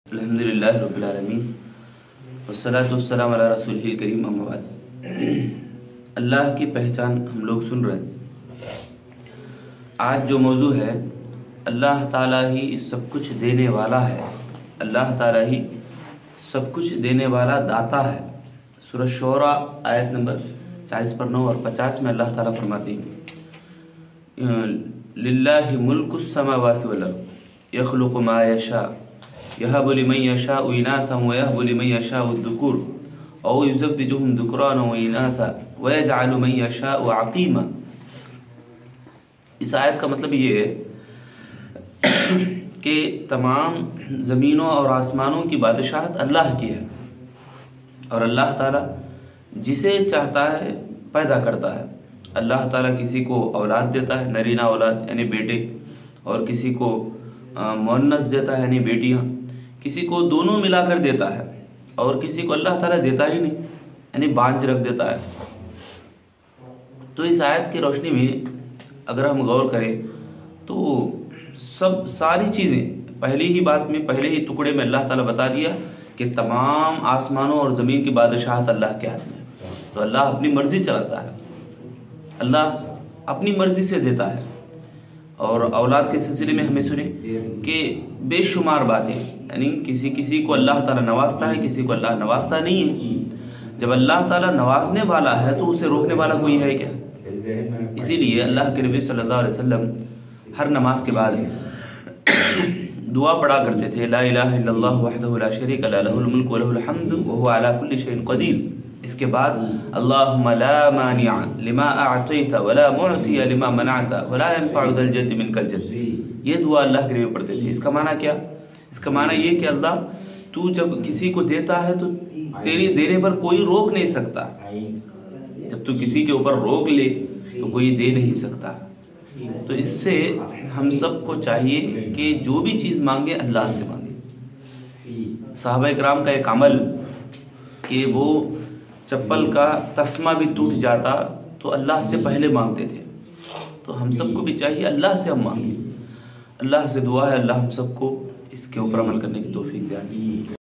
Aqeedah Class 14 Topic Allah Hi Sab Ka Data Hai By Abdul Mujeeb Umri
Markaz Masjid E Tauheed Jamat E Ahle Hadees Mukundar Gadda Rajampet